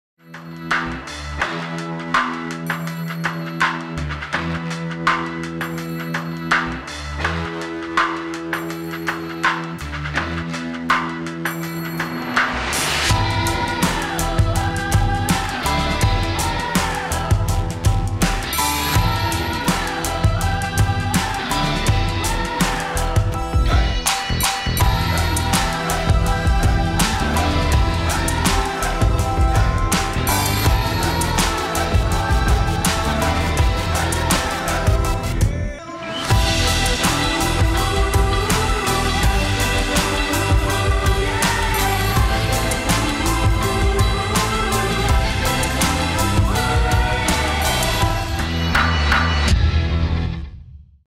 2016 UCI Track Cycling World Championships – Track Centre Experience
The Platinum Package at the 2016 UCI Track Cycling World Championships was located in the centre of the track offering exclusive hospitality guests unrivalled views of the racing action, right in the heart of the event. At STH Japan we provide unique hospitality experiences at top world class sporting events. for more information.